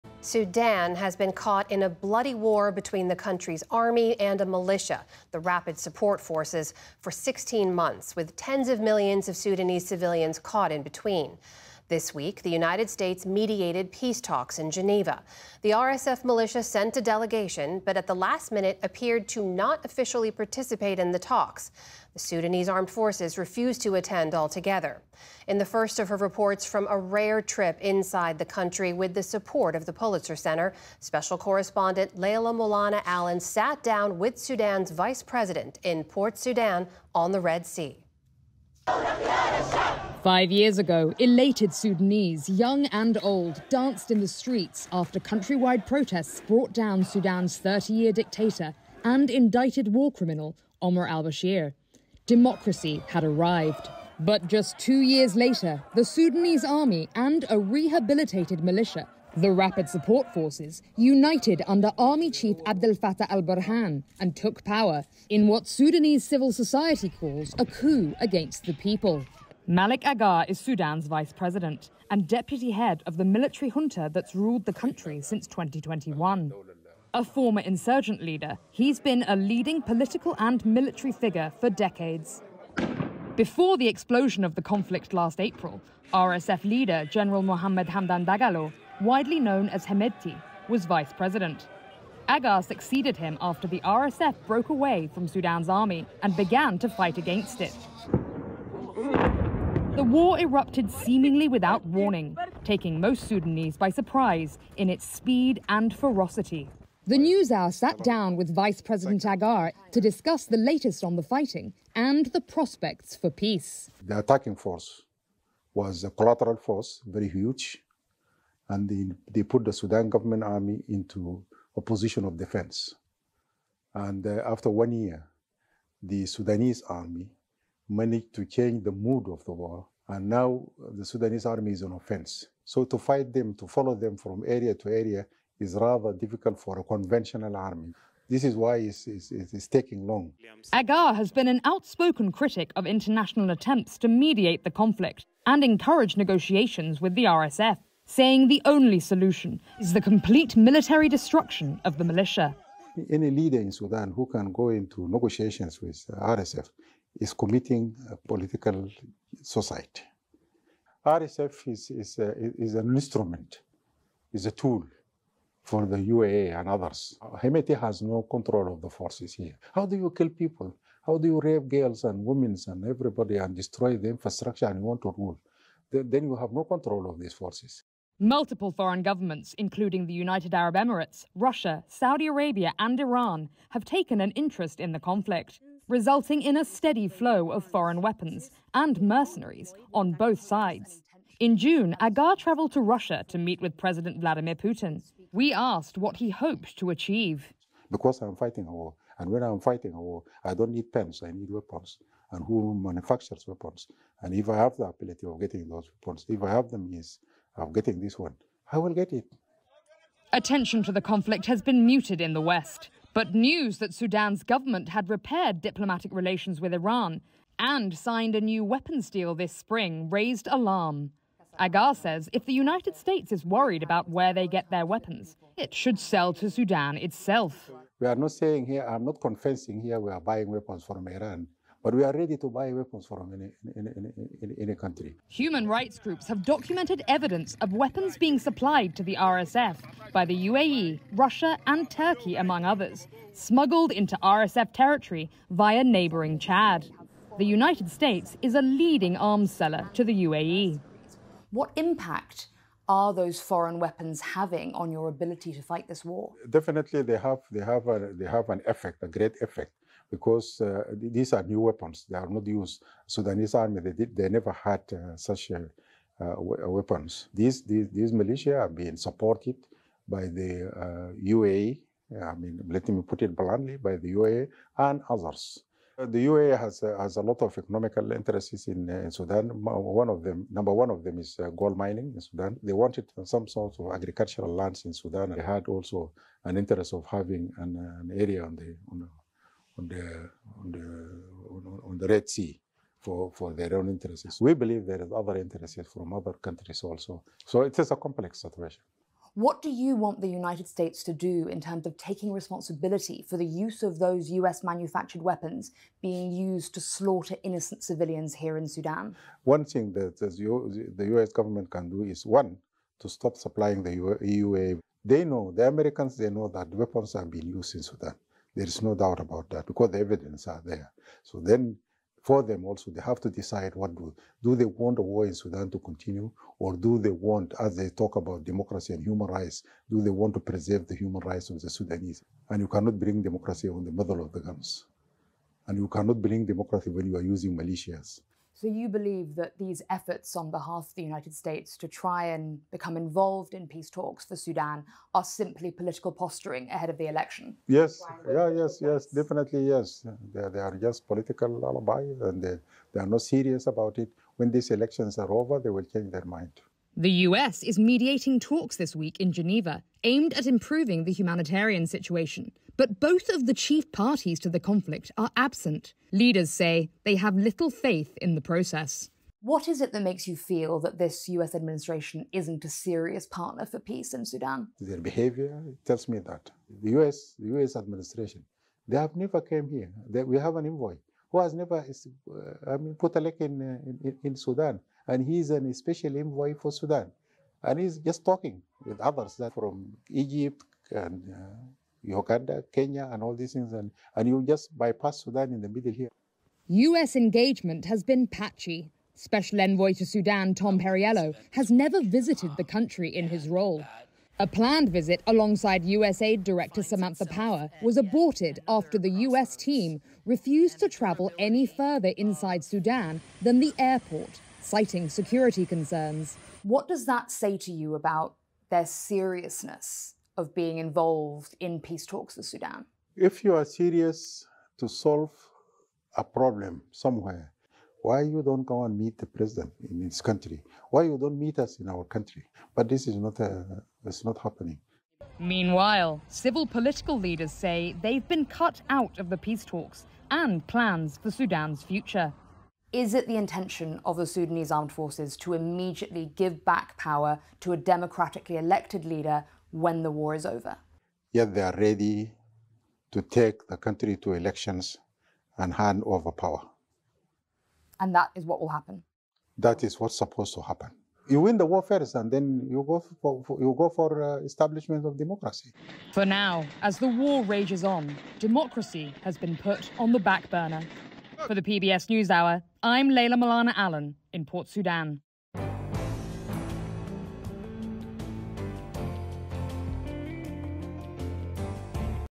Amid brutal civil war, Sudan's VP defends refusal to participate in U.S.-led peace talks
Sudan has been caught in a bloody war between the country’s army and a militia for 16 months with tens of millions of civilians caught in between. This week, the United States attempted peace talks in Geneva, but the Sudanese armed forces refused to attend. With the support of the Pulitzer Center, special correspondent Leila Molana-Allen discussed the war with Sudanese Vice President Malik Agar.